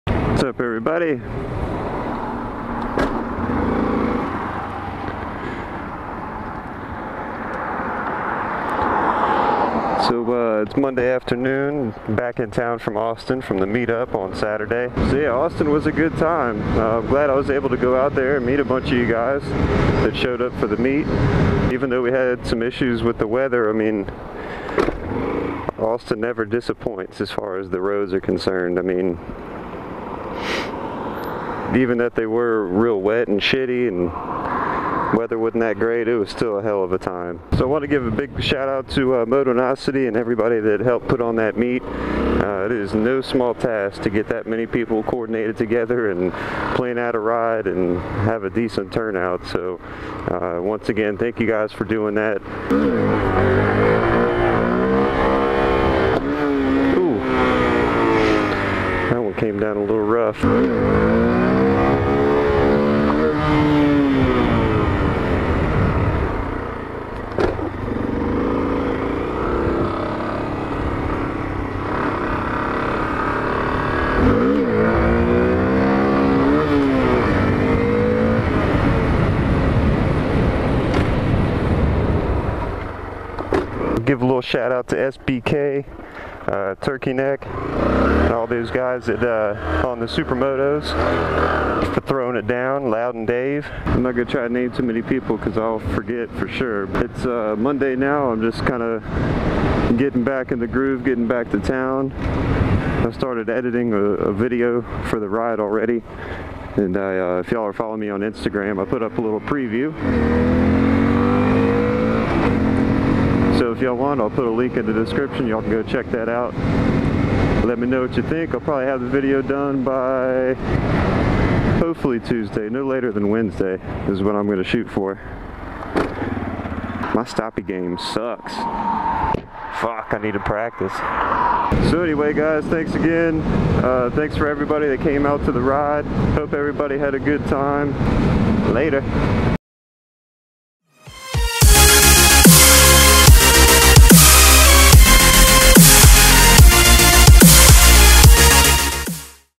Texas Moto Meet Recap
Thanks to everyone involved in putting on the Texas Meetup. I had a great time and met TONS of great people. See y'all at the next one!

Peep the Goons..